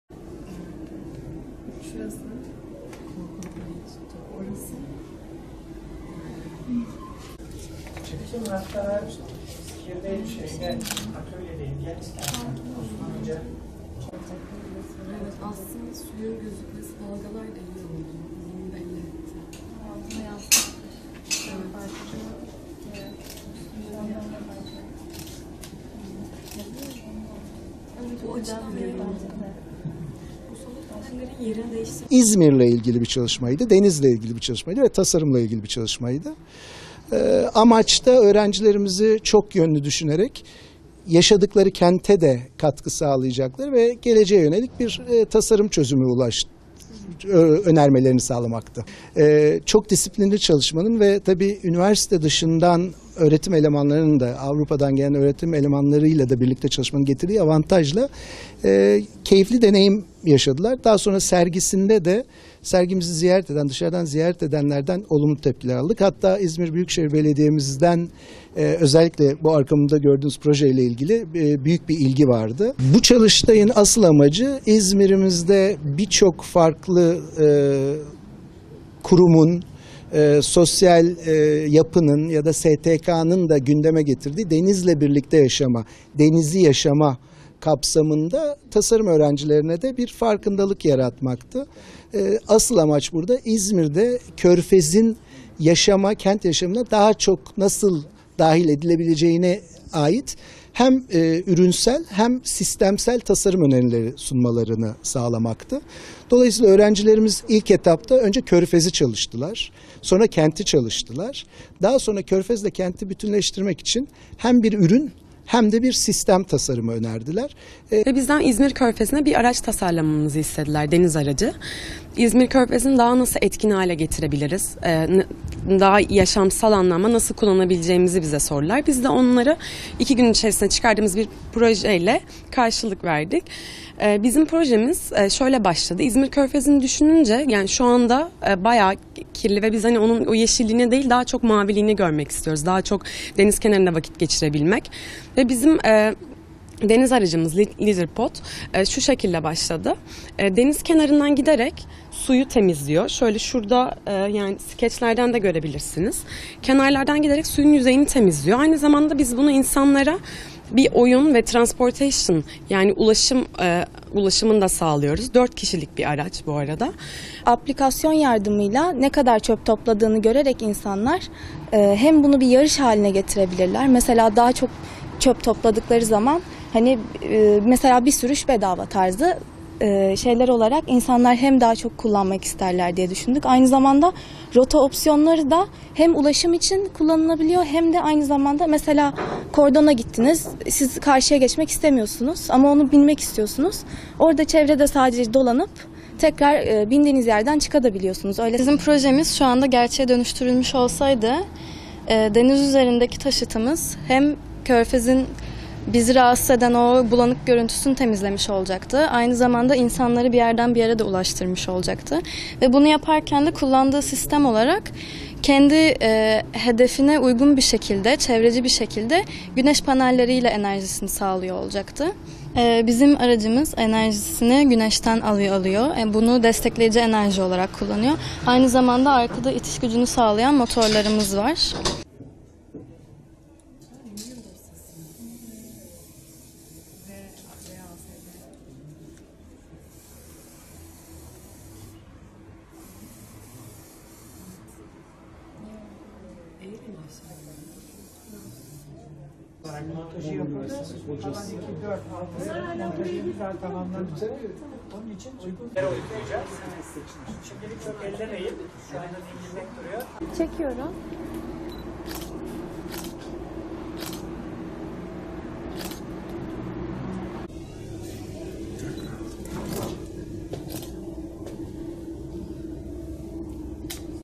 Öğrencilerden Körfez’i temizleyen taşıt
İç mimarlık öğrencileri İzmir Körfezi’ni temizleyen taşıt tasarladı